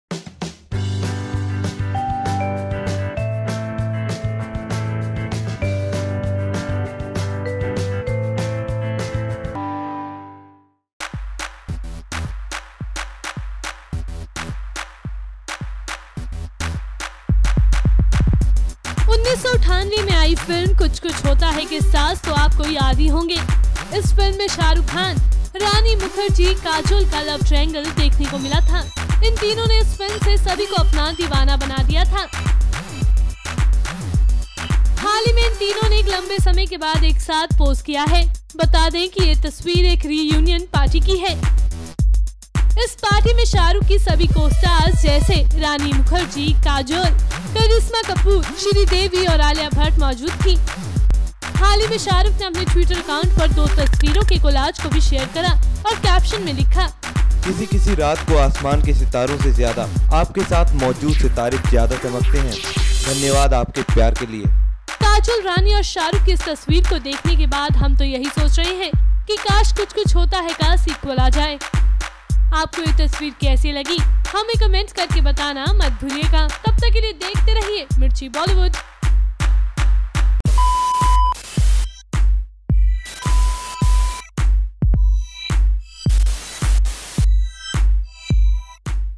Kuch Kuch Hota Hai Reunion :फिर मिले Shah Rukh Khan, Kajol and Rani Mukerji
Shah Rukh Khan poses with Kajol, Rani Mukerji, Sridevi, Alia Bhatt and Karisma Kapoor for a picture.The picture wherein he is posing for a selfie with Kajol and Rani Mukeji will take you back to the times of Kuch Kuch Hota Hai and will make you wish to see the trio once again sizzle on the screen.